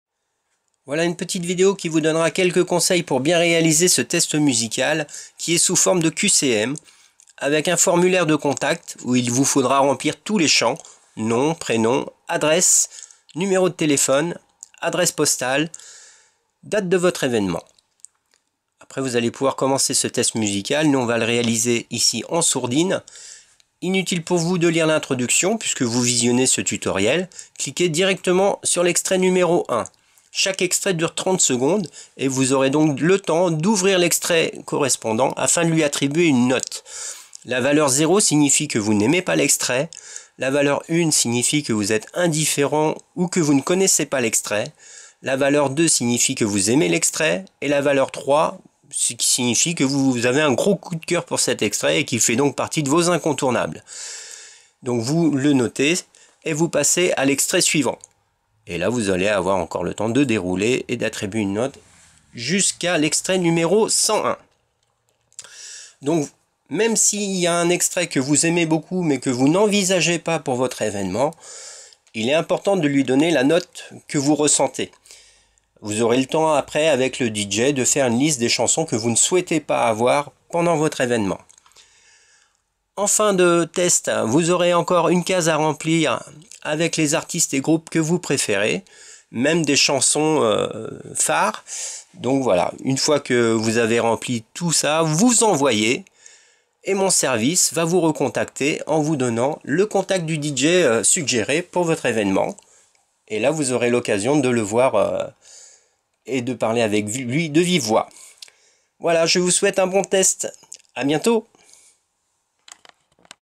Tutoriel test musical - DJ-SERVICE-44
Démo des dessins et animations contenu dans la carte sd d'origine